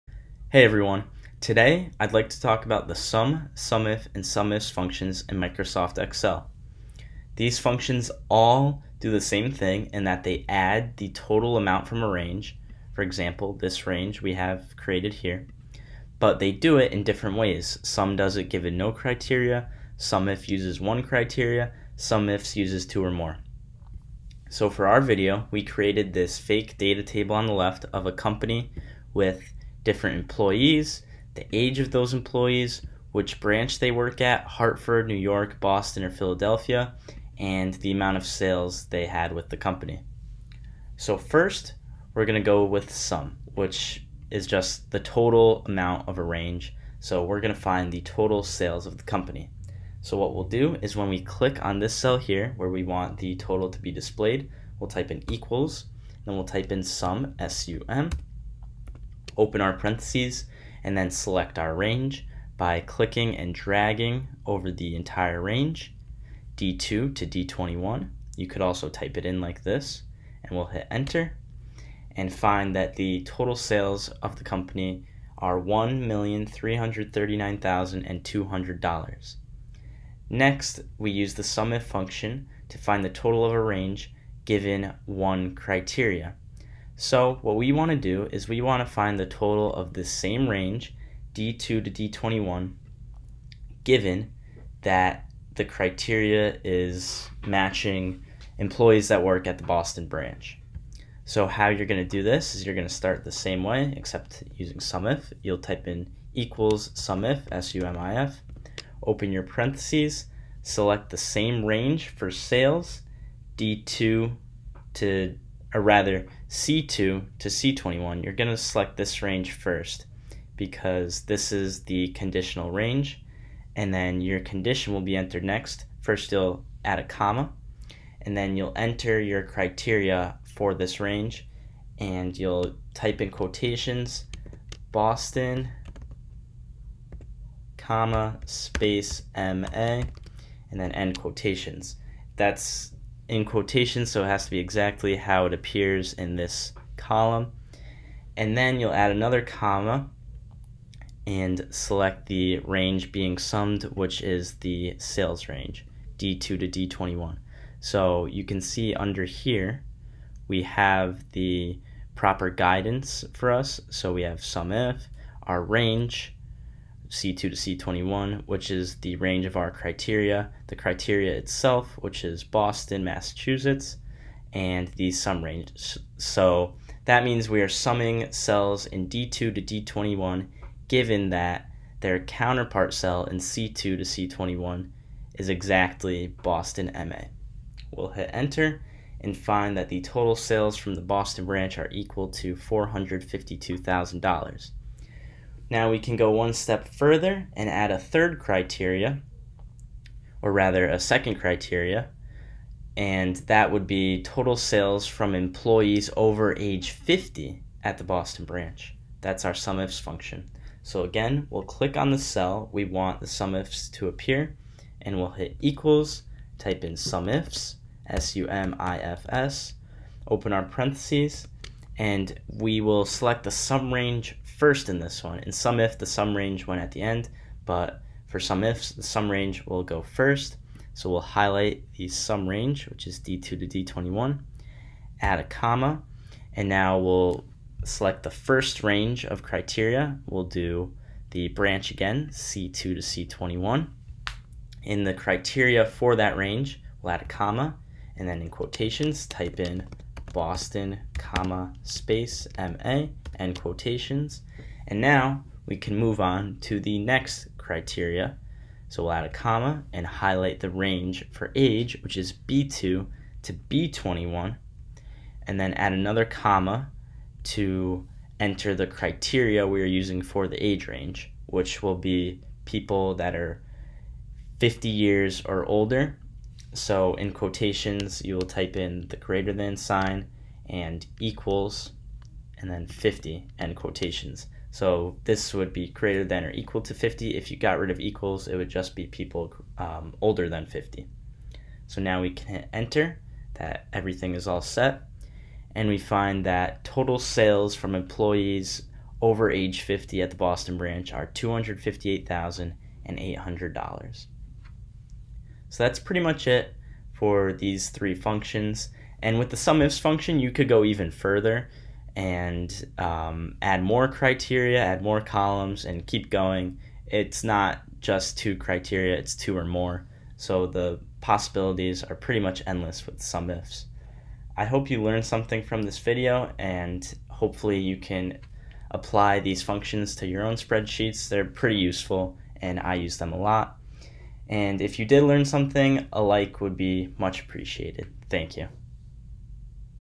Summing Values Based on One Or More Criteria In Microsoft Excel
SUM: The SUM function allows one to find the total sum of a range of values.

SUMIF: The SUMIF function allows one to find the total sum of a range of values based on a criteria.

SUMIFS: The SUMIFS function allows one to find the total sum of a range of values based on one or more criteria.